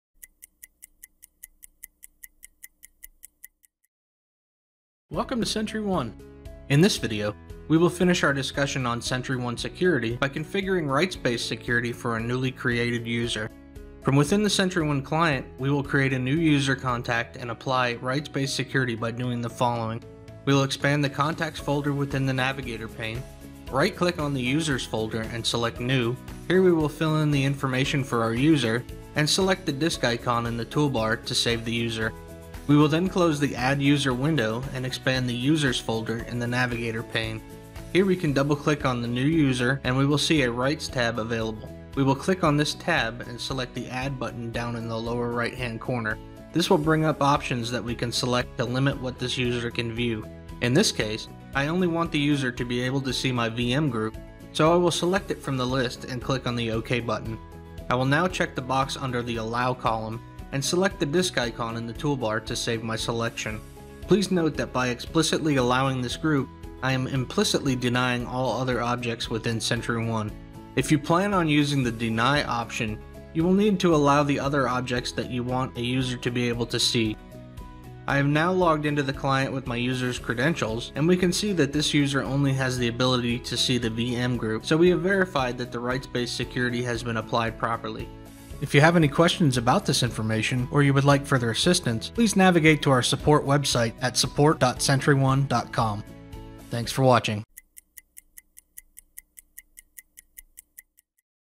Rights Based Security
This video concludes our discussion on SentryOne Security by setting up Rights Based Security for our new user.